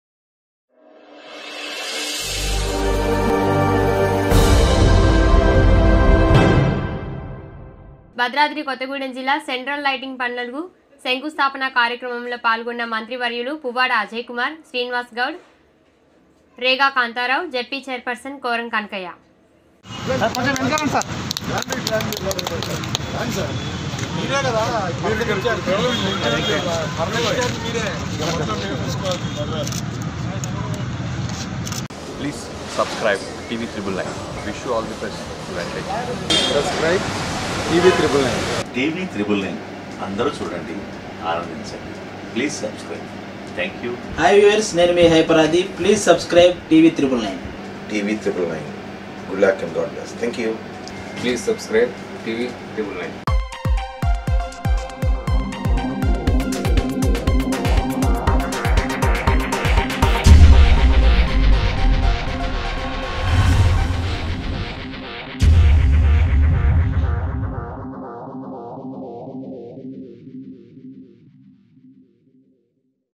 భద్రాచలం పట్టణంలో సెంట్రల్ లైటింగ్ పనులకు శంకుస్థాపన కార్యాక్రమంలో పాల్గొన్న మంత్రి వర్యులు
భద్రాచలం పట్టణంలో సెంట్రల్ లైటింగ్ పనులకు శంకుస్థాపన కార్యాక్రమంలో పాల్గొన్న మంత్రి వర్యులు శ్రీ.పువ్వాడ అజయ్ గారు,శ్రీనివాస్ గౌడ్ గారు,విప్ రేగా కాంతారావు గారు,జడ్పీ చైర్మన్ గౌ,,శ్రీ.కోరం కనకయ్య గారు